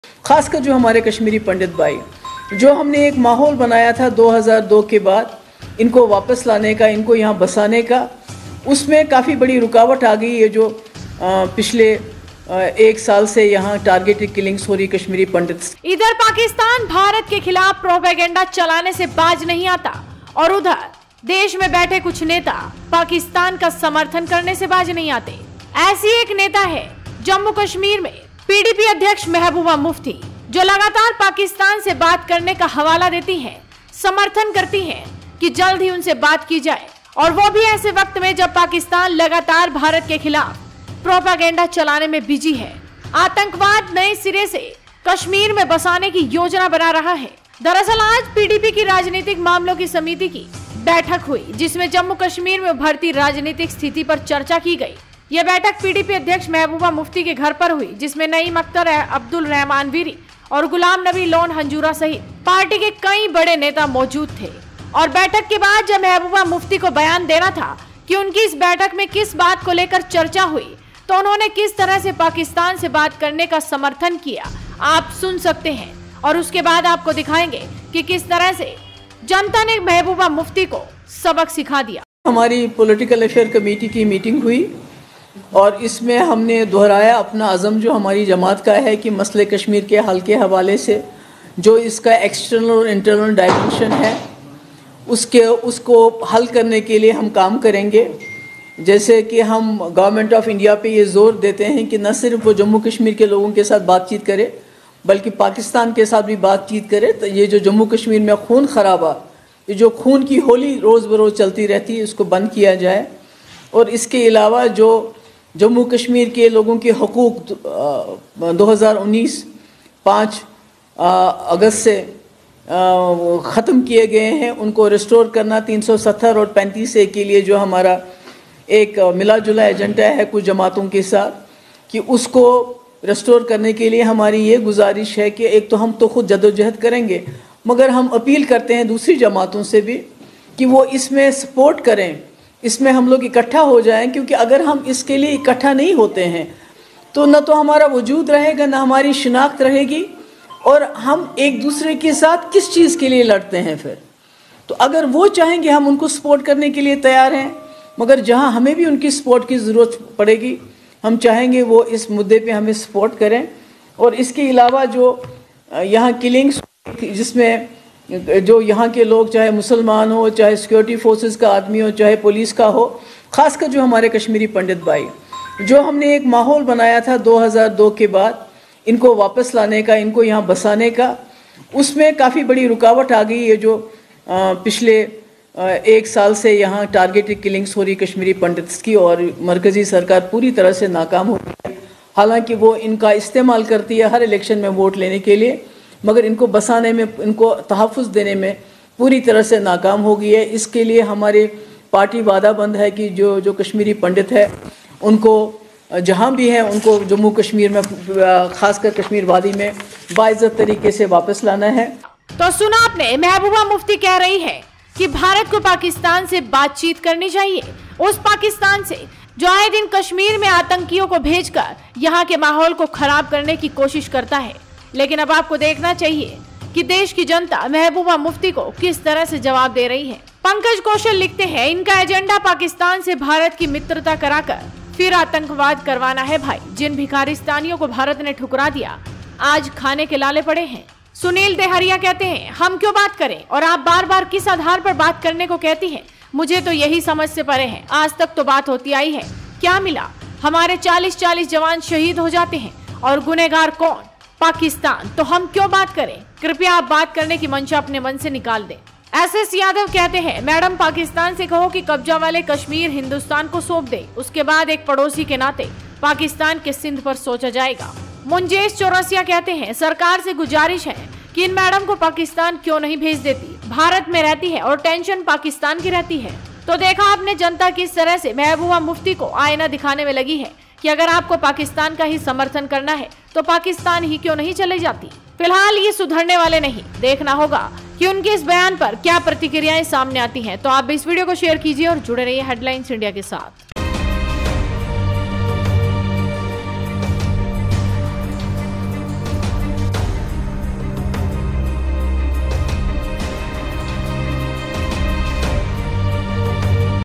महबूबा बोली कश्मीर के हल के लिए BJP पाकिस्तान के साथ बातचीत करें | Headlines India
महबूबा बोली कश्मीर के हल के लिए BJP पाकिस्तान के साथ बातचीत करें